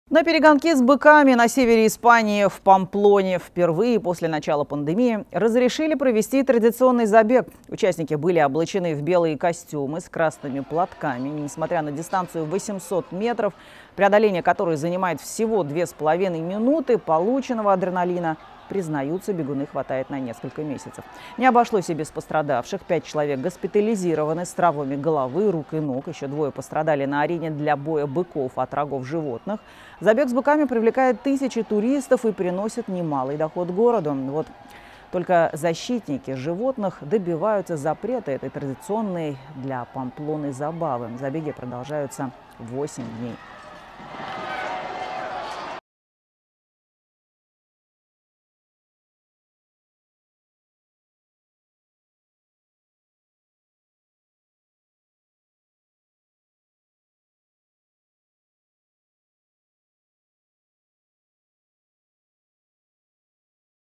В результате забега с быками в Испании пострадало 5 человек
На севере Испании, в Памплоне, впервые после начала пандемии, разрешили провести традиционный забег быков. Участники были облачены в белые костюмы с красными платками. Несмотря на дистанцию в 800 метров, преодоление которой занимает всего две с половиной минуты, полученного адреналина, признаются бегуны, хватает на несколько месяцев. Не обошлось без пострадавших.